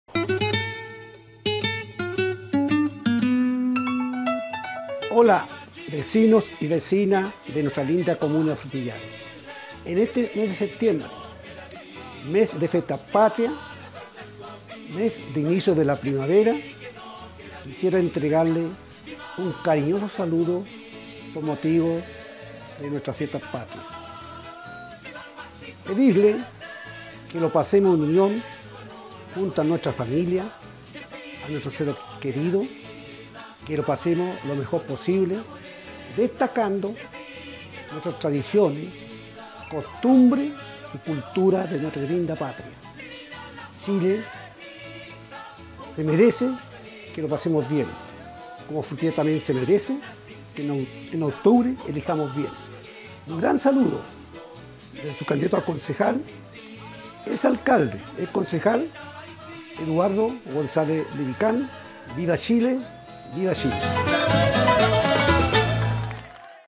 EDUARDO GONZÁLEZ LEVICÁN saluda a la Patria en VisionTv Radio
Saludo a la comuna de Frutillar de Eduardo González Levicán, candidato a Concejal por Frutillar en estas Fiestas Patrias 2024.

Gentileza de VisionTv Radio.
Sigue nuestra página en Facebook como VisionTv Frutillar.